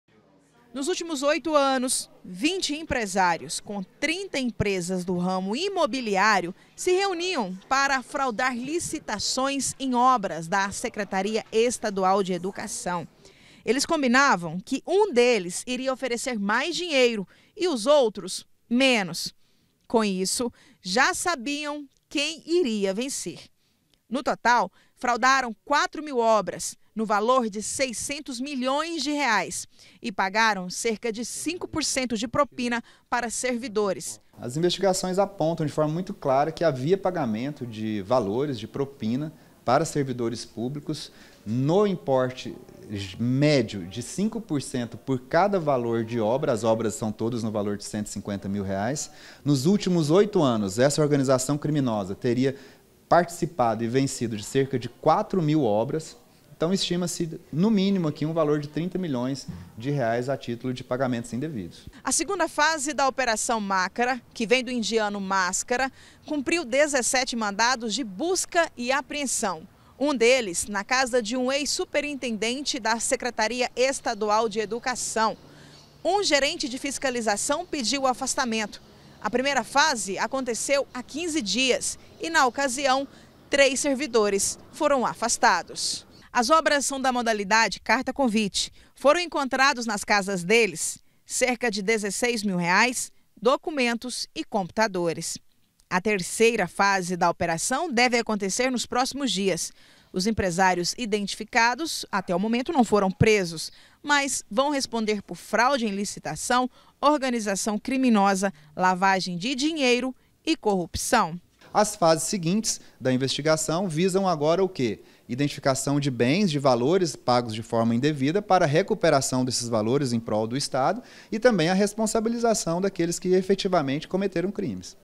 FRAUDE EM LICITAÇÃO: GRUPO É SUSPEITO DE RECEBER 30 MILHÕES EM PROPINAS
A Polícia Civil cumpriu hoje, 17 mandados de busca e apreensão em empresas de construção e na casa de servidores públicos. São acusados de fraude em  licitações da secretaria estadual de educação. Em oito anos teriam sido pagos, só em propina, mais de 30 milhões de reais.